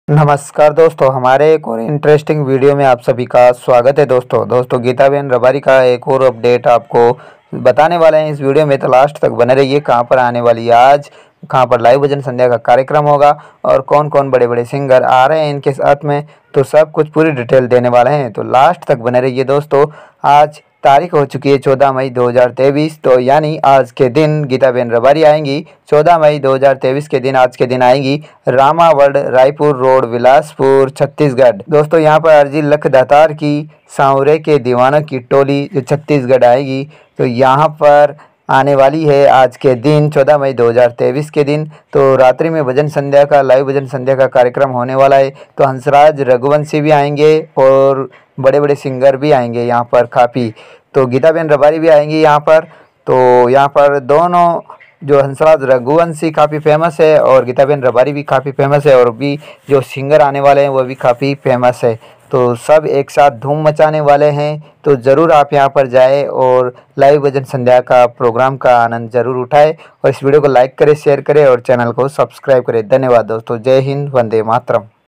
गीता रबारी, हंसराज रघुवंशी पहली बार मंच पर | Geeta Rabari Live Bilaspur Chhattisgarh
गीता रबारी, हंसराज रघुवंशी पहली बार मंच पर, भीड़ संभालना मुश्किल | Geeta Rabari Live Bilaspur